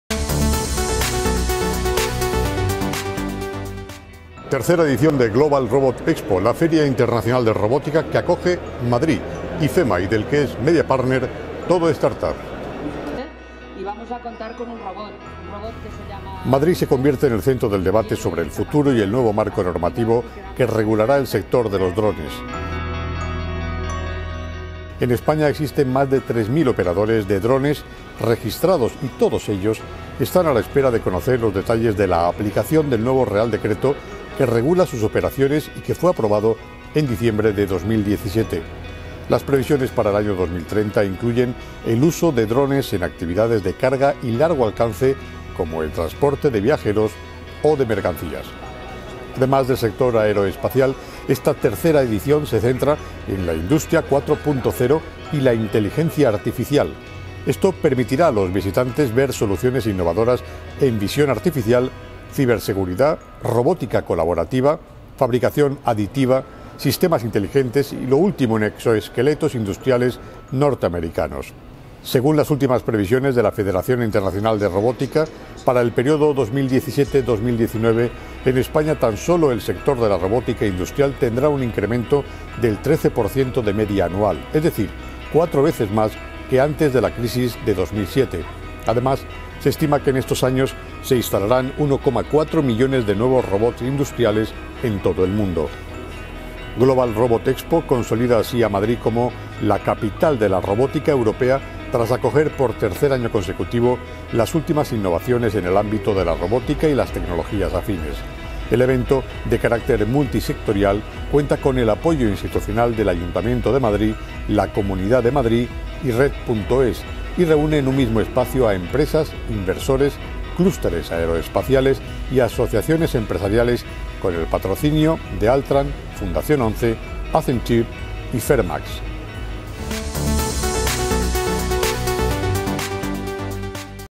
TodoStartups - III Edición de Global Robot Expo, la feria internacional de robótica
Madrid se convierte en el centro del debate sobre el futuro y el nuevo marco normativo que regulará el sector de los drones. En España, existen más de 3.000 operadores de drones registrados y , todos ellos, están a la espera de conocer los detalles de la aplicación del nuevo Real Decreto que regula sus operaciones y que fue aprobado en diciembre de 2017. las previsiones pare el año 2030 incluyen el uso de drones en actividades de carga de largo alcance como el transporte de viajeros o de mercancías.

Además el sector aeroespacial, esta tercera edición se centra en la Industria 4 y la Inteligencia Artificial. Esto permitirá a los visitantes ver soluciones innovadoras en visión artificial, ciberseguridad, robótica colaborativa, fabricación aditiva, sistemas inteligentes y lo último en exoesqueletos industriales norteamericanos.